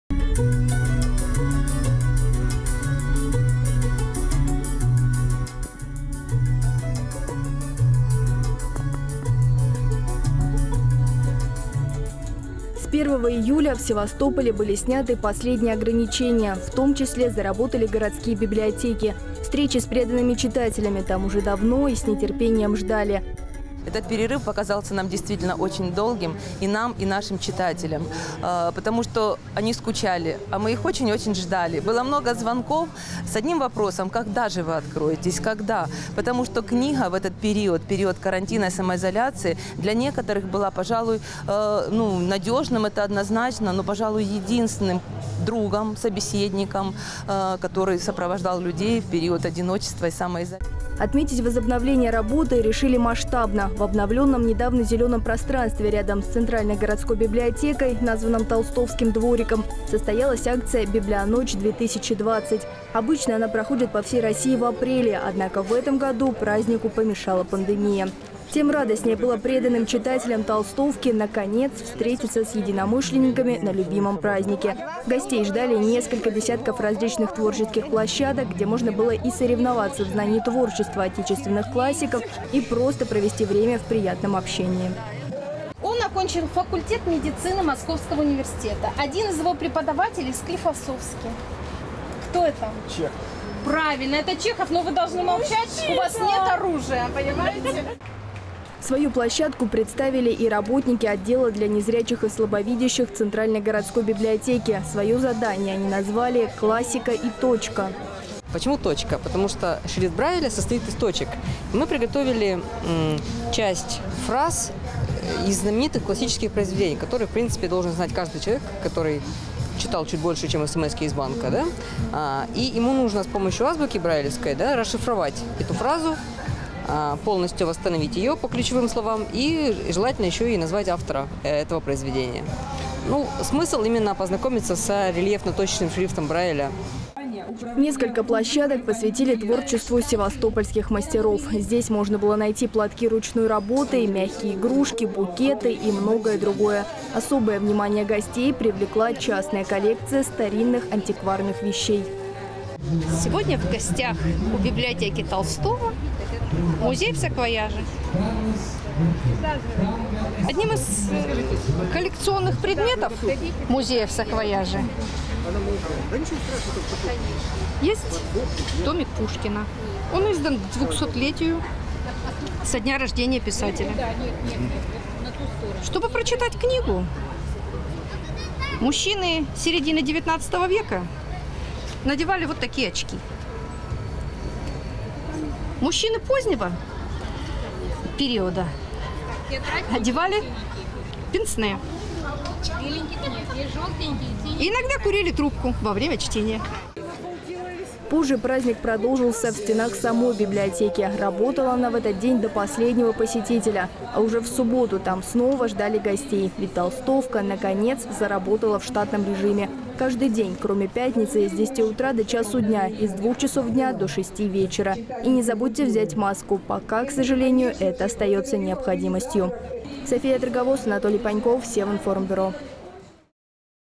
В библиотеке имени Толстого в Севастополе прошла Библионочь-2020
В пятницу в библиотеке имени Льва Николаевича Толстого состоялась Библионочь-2020. Это первое масштабное мероприятие после долго перерыва, вызванного карантином. Для гостей праздника подготовили множество творческих площадок, где можно было проверить свои знания в области литературы, узнать много нового и даже выиграть сладкие призы. Как прошла первая встреча библиотеки с любимыми читателями – в нашем следующем материале.